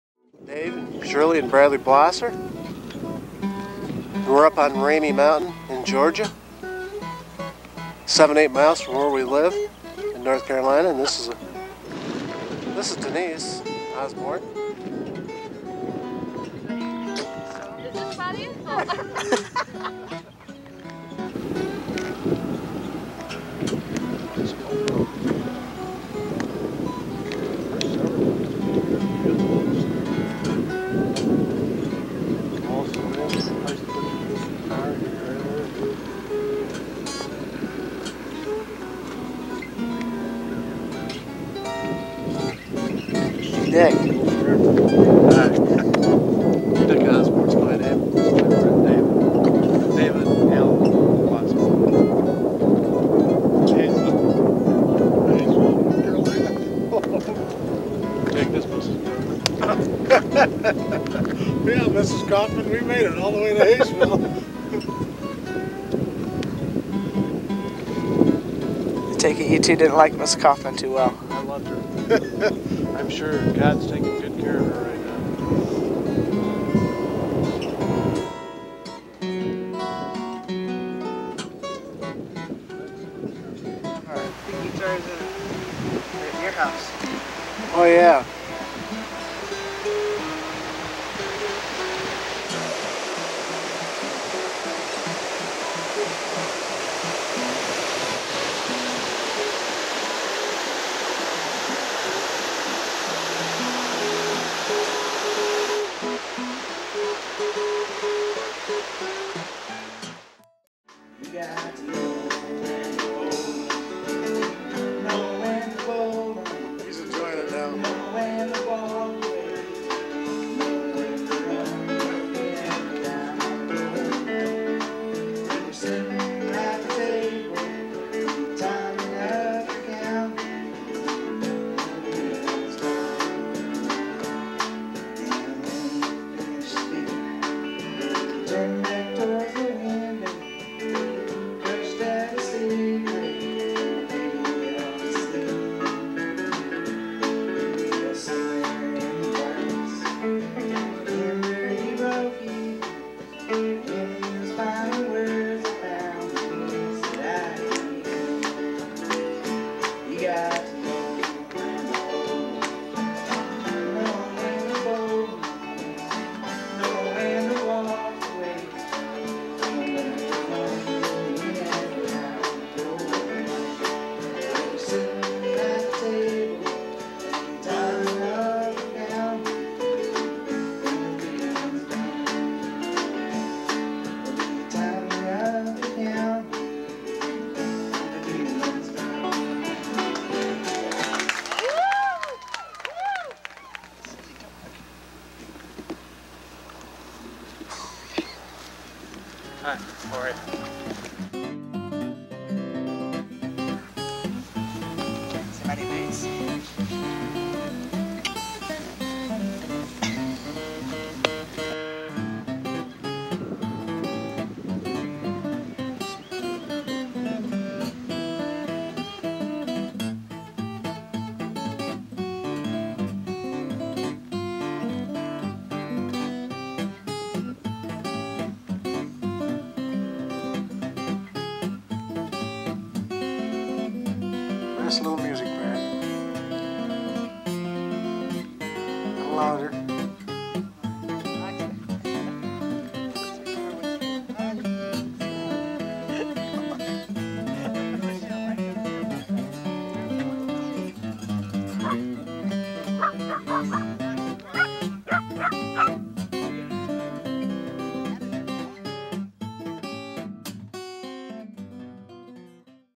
Hayesville '93
Family vacation to Hayesville, NC in '93. Osborne show Blossers a good time.